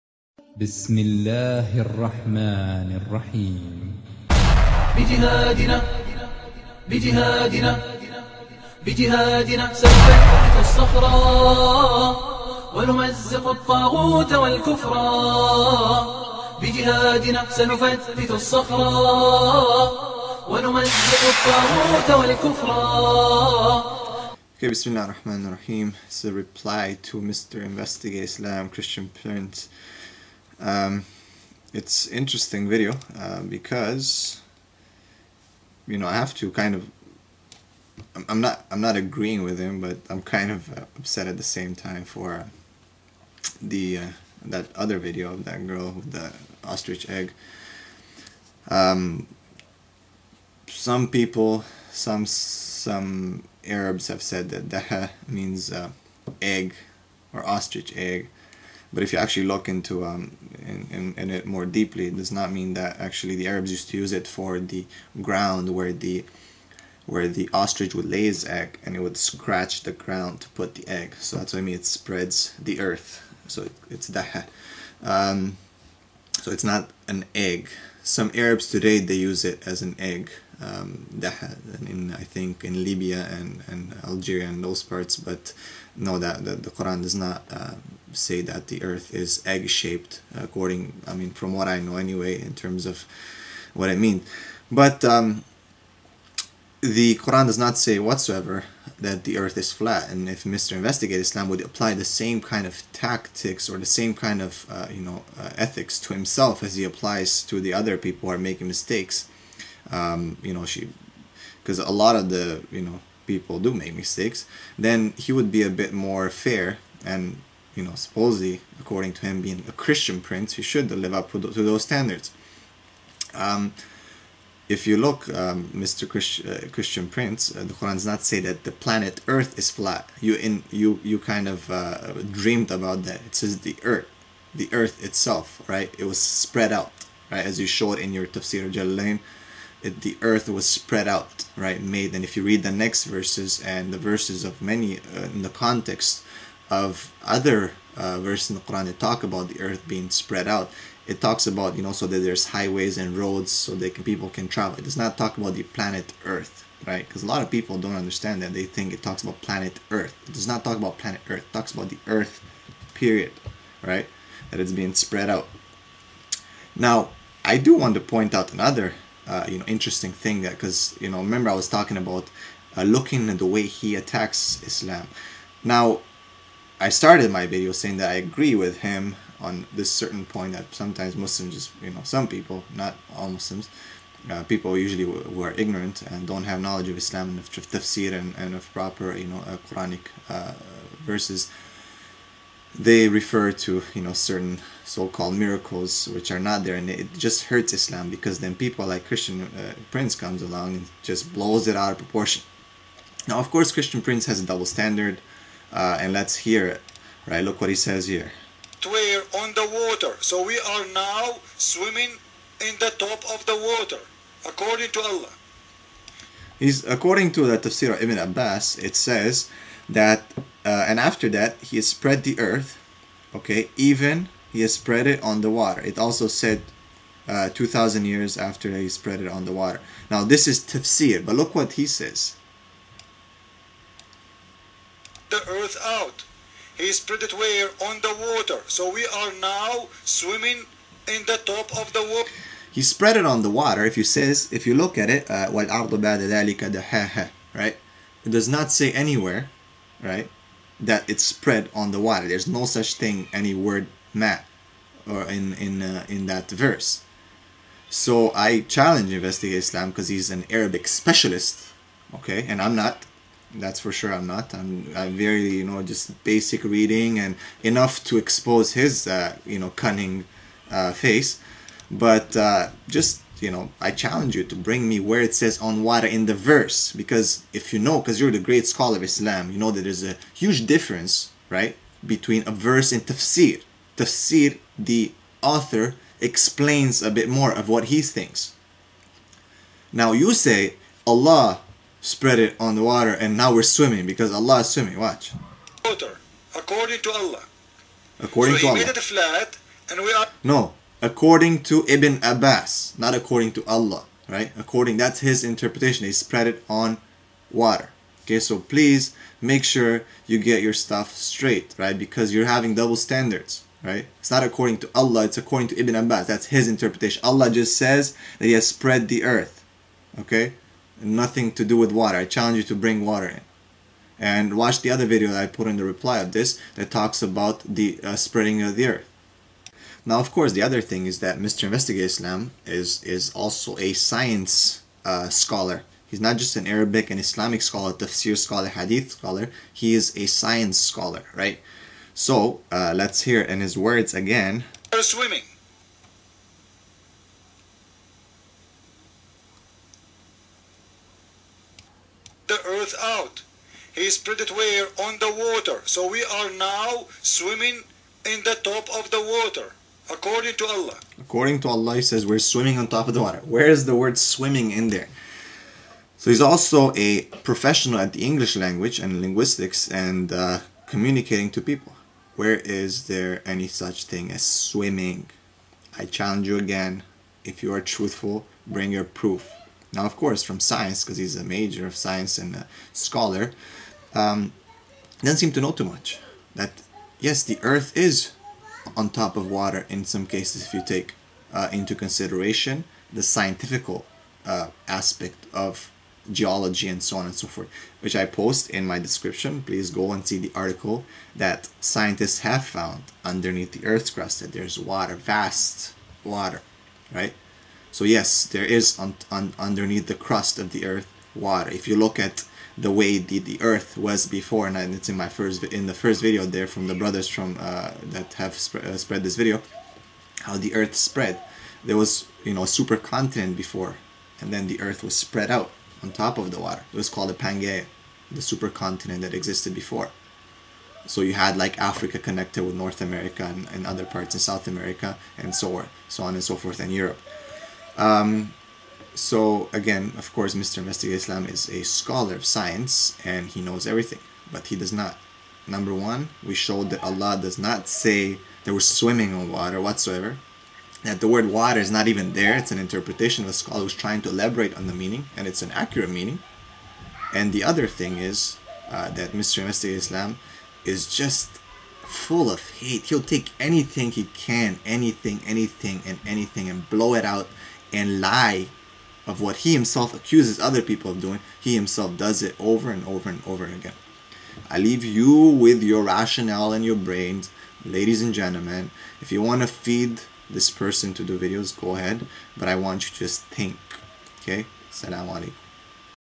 Earth like egg?
Please see the following article and realize that the description of Ibn Abbas was correct, and that Christian Prince is once again wrong